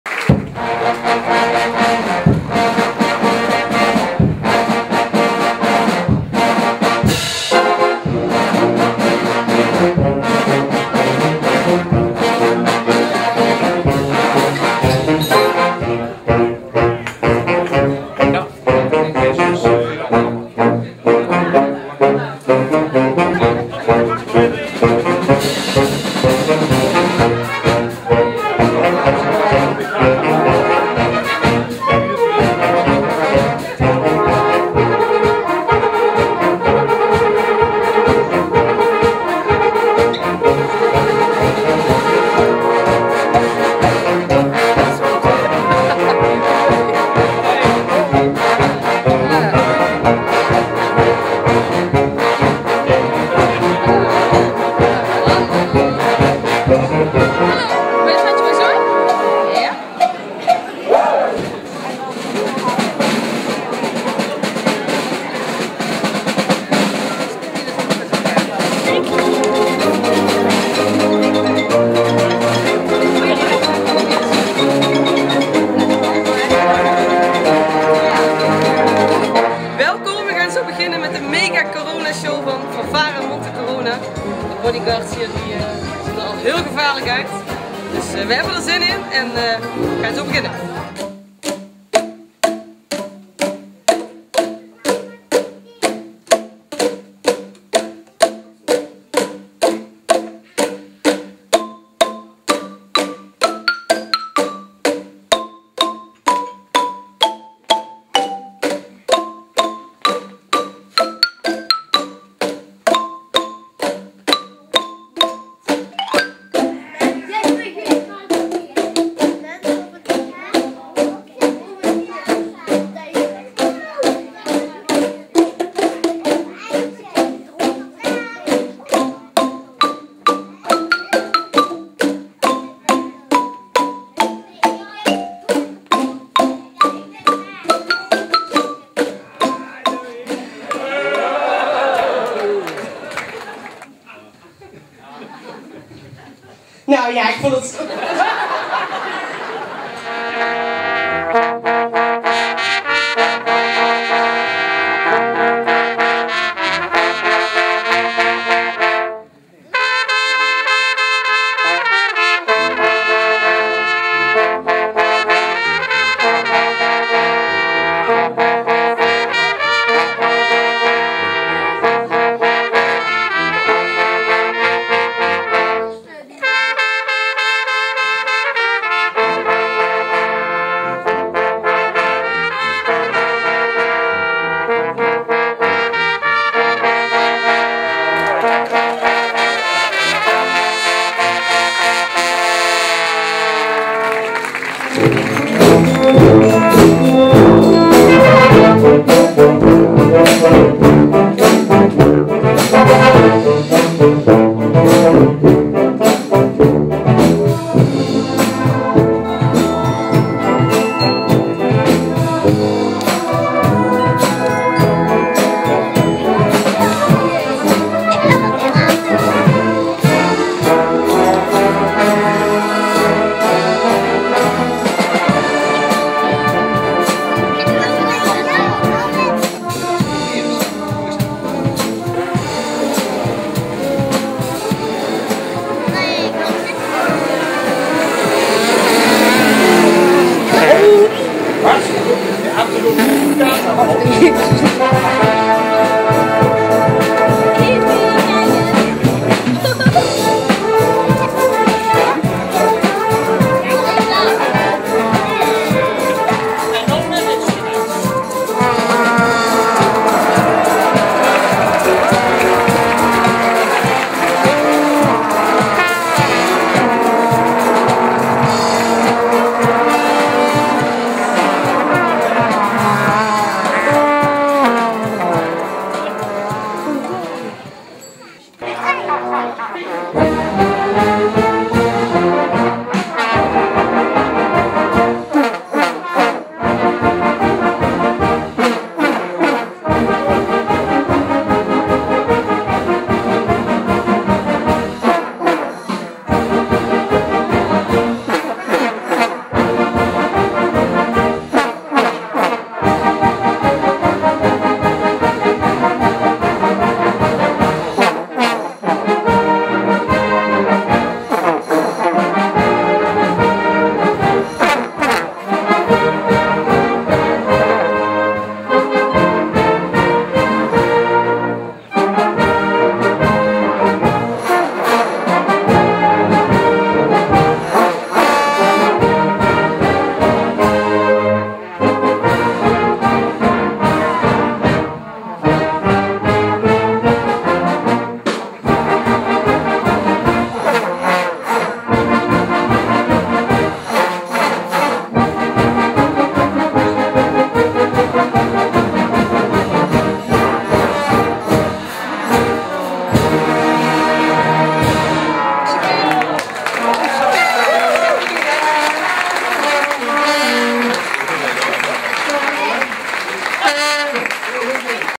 Limonadeconcert 2 16 movie
Monte Corona pakte tijdens dit Limonadeconcert GROOTS uit. Alle VIPS werden door Bodyguards begeleid, kregen heerlijke Ranja en cake en zaten prinsheerlijk vooraan op het concert. De Special Act, alias Beethoven, pakte uit door de kids mee te laten spelen op ECHTE INSTRUMENTEN. Zo werd het MEGA CORONA toen iedereen meespeelde met het orkest. Kronenberg Toetert !